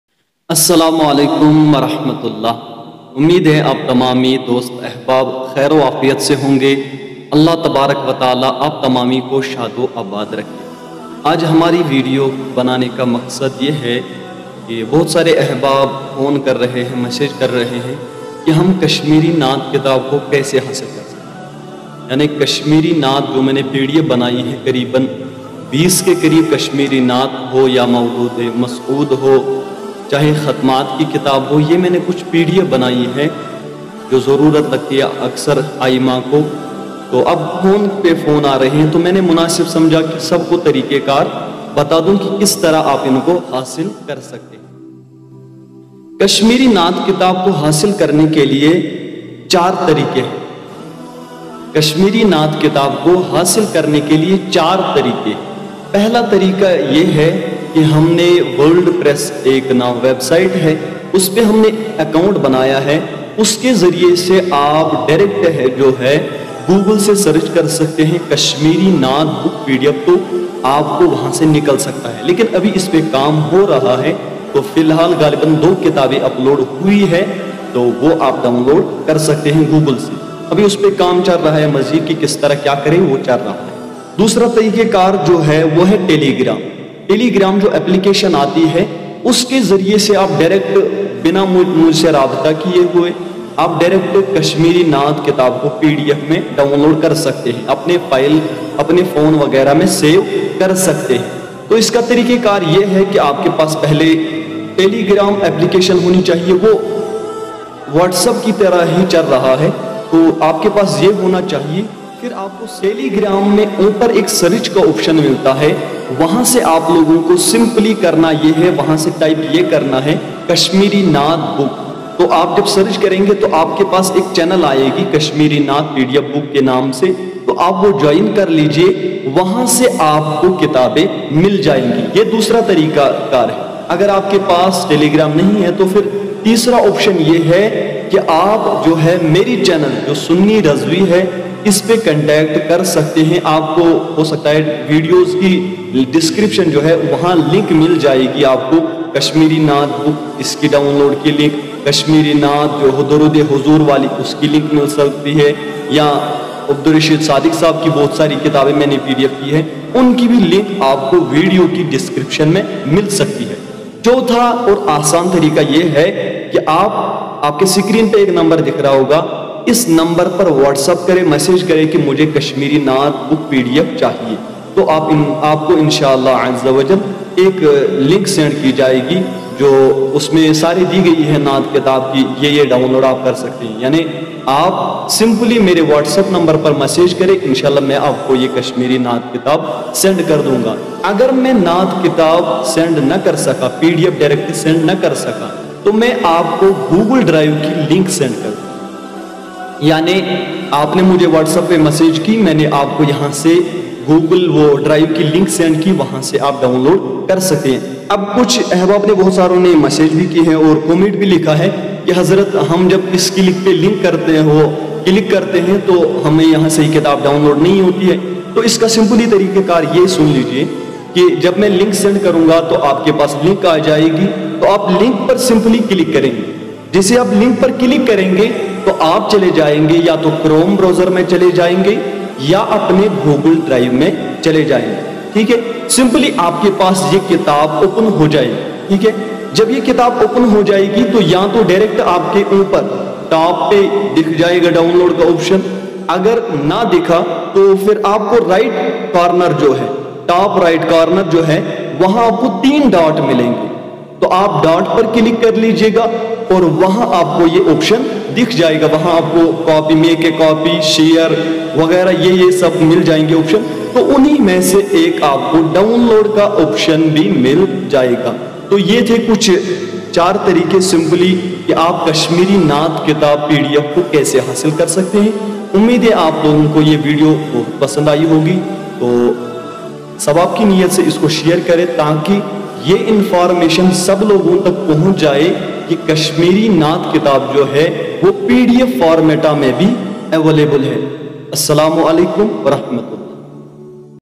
How We Can Download Kashmiri Naat Pdf Book | Mushtaq Raza Attari.
Video Title :- How We can down Kashmiri Naat Pdf Book کشمیری نعت کتاب pdf کیسے حاصل کریں

Speaker :- Mushtaq Raza Attari

Video :-  SUNNI RAZVI YouTube channel

گوگل ڈرائیو اور ٹیلی گرام سے ڈاؤنلوڈ کرنے کا طریقہ ۔ ویڈیو کو دیکھیں
Telegram Ya Google Drive Sa Kysa Download Kara Step by step Watch This Video ❗

Join us For more updates on ⤵

*اسلام عليكم ورحمة اللة وبركاته*
 *اگر آپ نعت رسول مقبول ﷺ کے خواہش مند ہیں
تو خوش ہو جائے آپ کےلئے کشمیری نعت کتاب ۔۔*
 *KASHMIRI NAAT PDF BOOKS*

1:- نور مدینہ کشمیری نعت رسول مقبول ﷺ کتاب ۔

8:-  رضائے بخشش کشمیری نعت کتاب  میرے استاد کی نعت کتاب ۔

9:- آتشکدہ عشق مولانا مشتاق احمد خان کی نعت کتاب ۔

10:- آتشکدہ عشق حصہ دوم

14:- مولود نبی ﷺ اور کشمیری نعت

15 جامعہ طیبہ از مولانا غلام رسول حامی صاحب

16 کبریت الاحمر اور اوراد قادریہ

17 :- ختمات شریف pdf

18 :- مدینک پوش نعت کتاب رشید صادق

19:- دعاء صبح مع کشمیری نعتیں

20:- نعت نبی صلی اللہ علیہ وسلم از پیر محمد یعقوب شاہ صاحب ( بلبل کولگامی )

21:- التجا بدربار رسول اللہ صلی اللہ علیہ
پیر محمد یعقوب شاہ صاحب بلبل کولگامی

22:- درود حضور کشمیری نعت کتاب